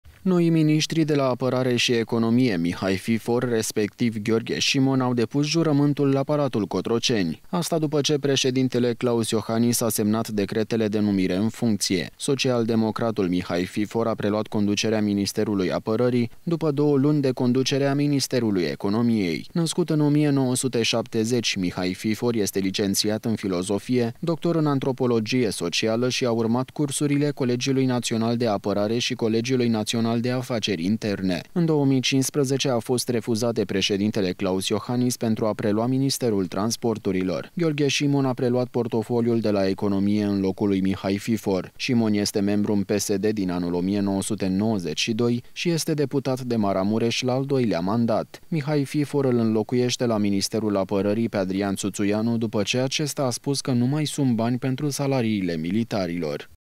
Noi numiri, la Apărare şi Economie
Noii miniştri de la Apărare şi Economie, Mihai Fifor, respectiv Gheorghe Şimon au depus jurământul la Palatul Cotroceni! Asta după ce președintele Klaus Iohannis a semnat decretele de numire în funcţie. Social-democratul Mihai Fifor a preluat conducerea Ministerului Apărării, după două luni de conducere a Ministerului Economiei.

Născut în 1970, Mihai Fifor este licenţiat în Filosofie, doctor în Antropologie Socială şi a urmat cursurile Colegiului Naţional de Apărare şi Colegiului Naţional de Afaceri Interne. În 2015, a fost refuzat de preşedintele Klaus Iohannis pentru a prelua ministerul Transporturilor. Gheorghe Şimon a preluat portofoliul de la Economie în locul lui Mihai Fifor. Șimon este membru PSD din anul 1992 şi este deputat de Maramureş, la al doilea mandat. Mihai Fifor îl înlocuiește la ministerul Apărării pe Adrian Țuțuianu după ce acesta a spus că nu mai sunt bani pentru salariile militarilor.